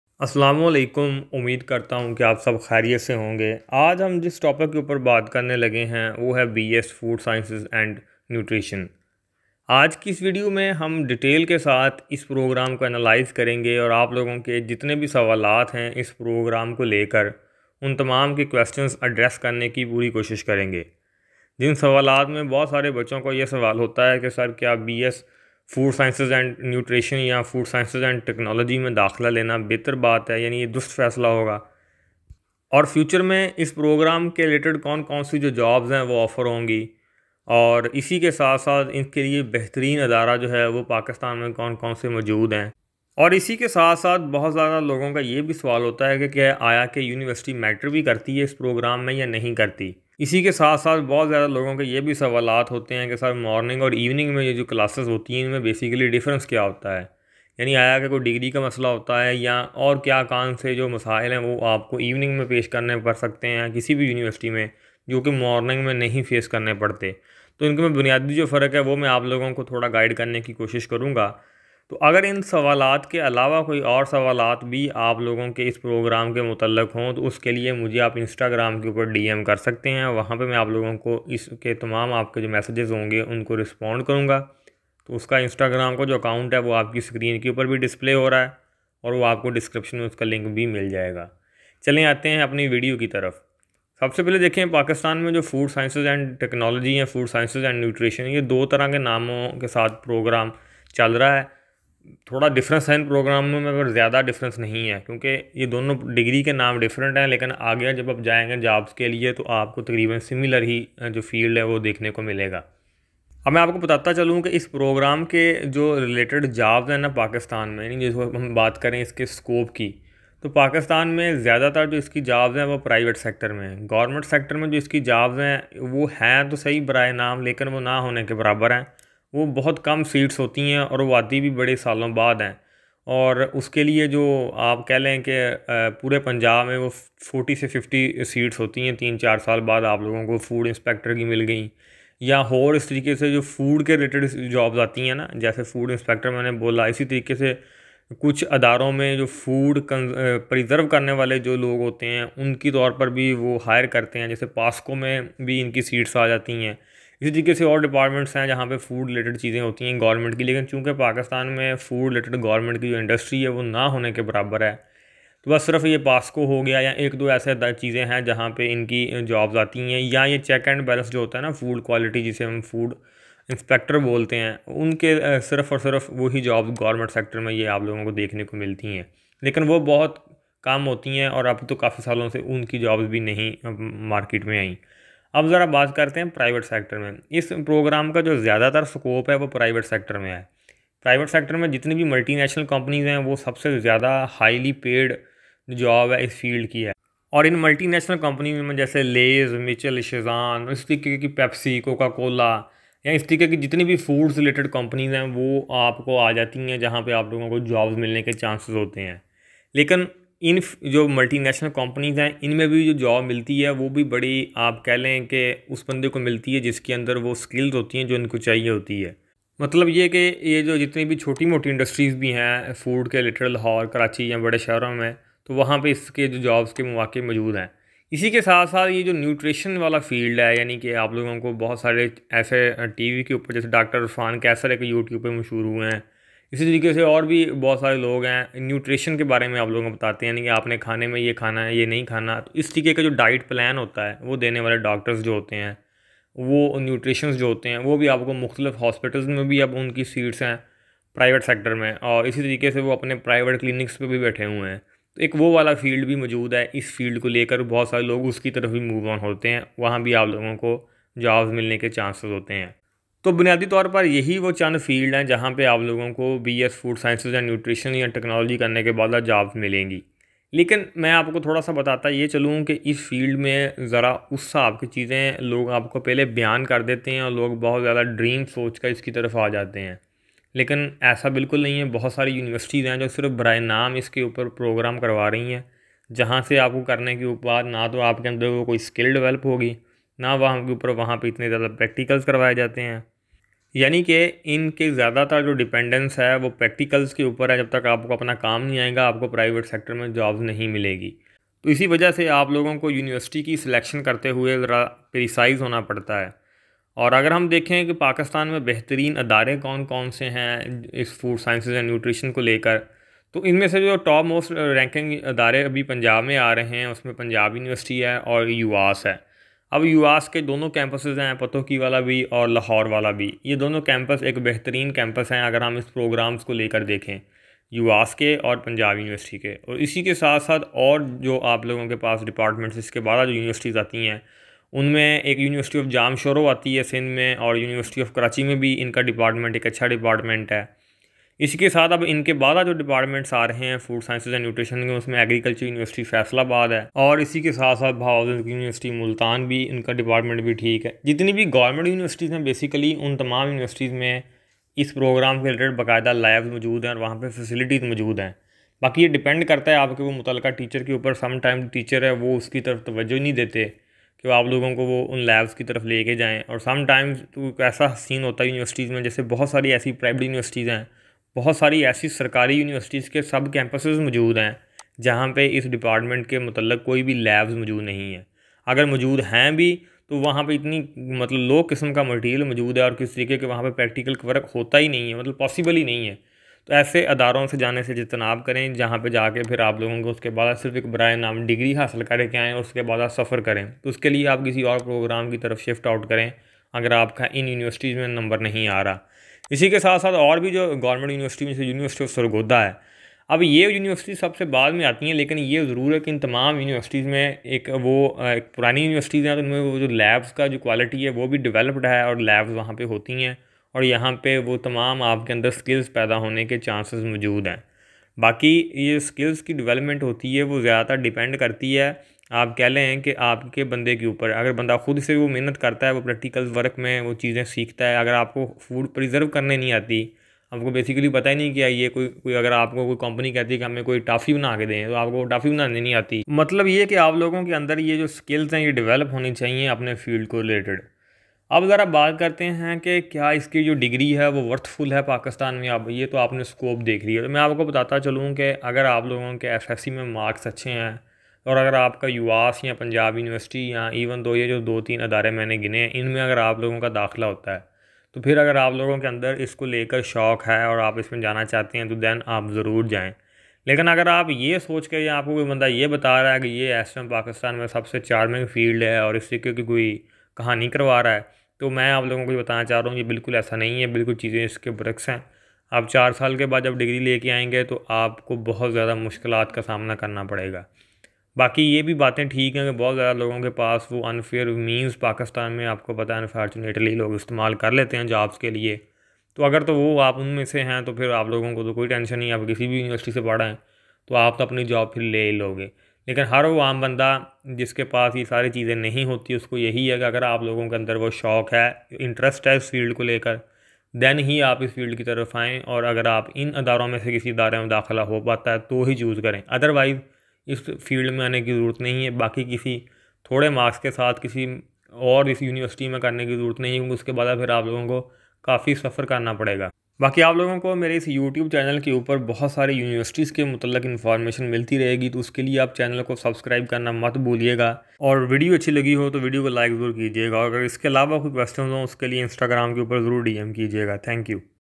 Best Universities For BS Food Science & Technology | Bs Food Science Scope in Pakistan 2023
is bs food science and technology is good degree in Pakistan? In this video what is the scope of bs food science and technology in Pakistan .